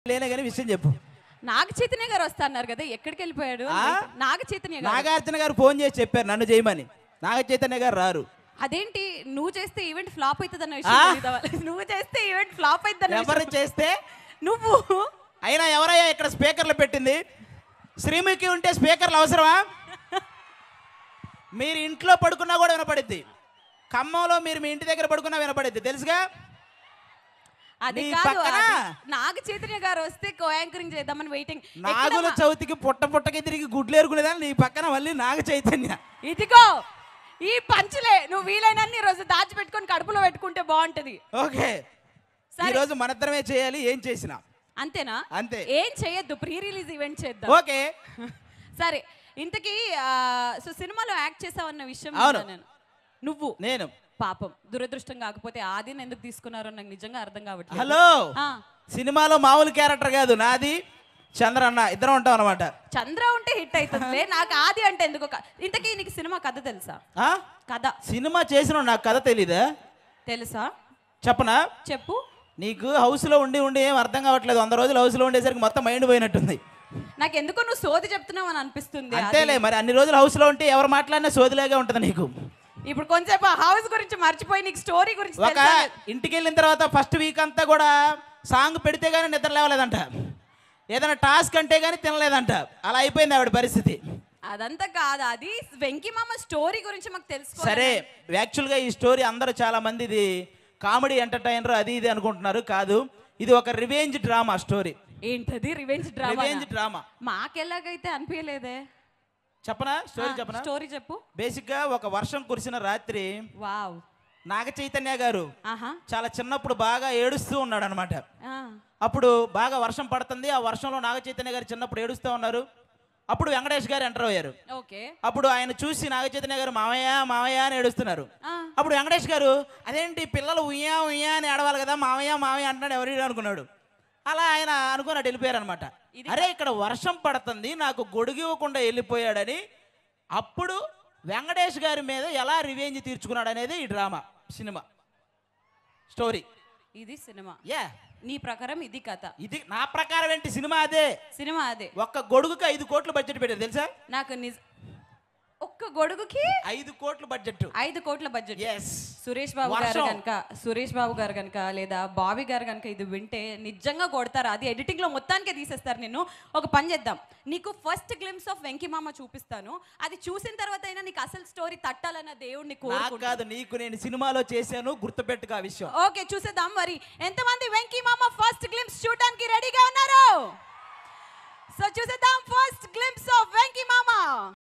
Hyper Aadi Funny comments on Sreemukhi | Hyper Aadi Making Fun on Anchor Sreemukhi
Hyper Aadi and Anchor Sreemukhi Making Fun in VenkyMama Pre Release Event